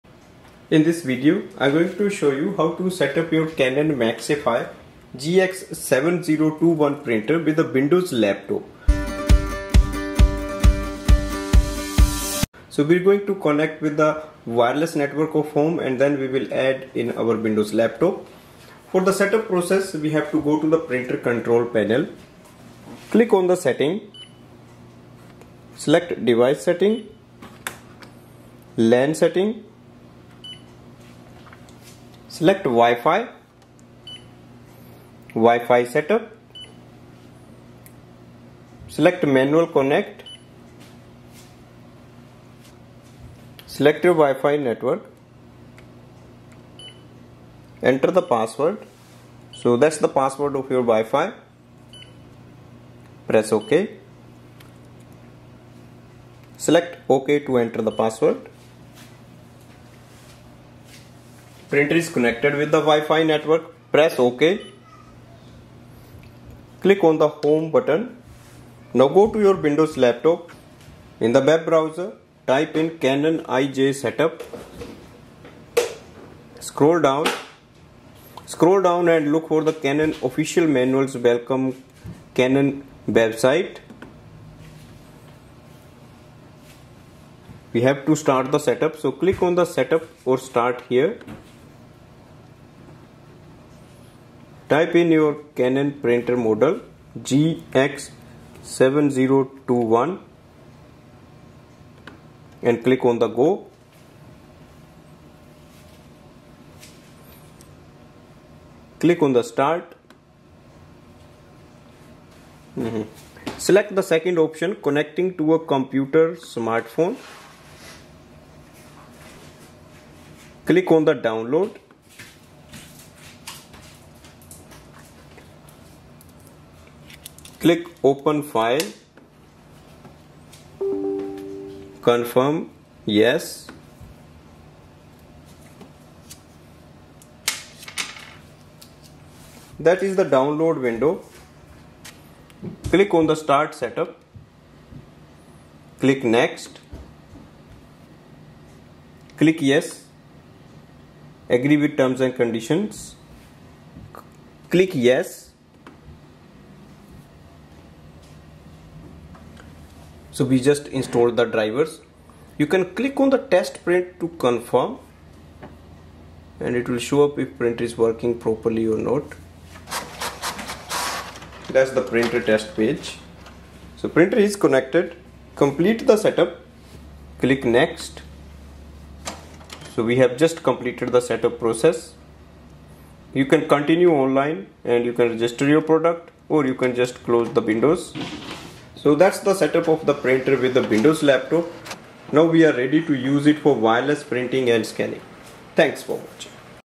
Canon Maxify GX7021 Setup Windows Laptop, Install Drivers, Connect To Wireless Network !!
This video reviews how to set up your Canon Maxify GX7021 All-in-one Printer, and add in the Windows laptop or PC, you can install in Windows XP, VISTA, 7, 8, 10 or 11, wireless setup review.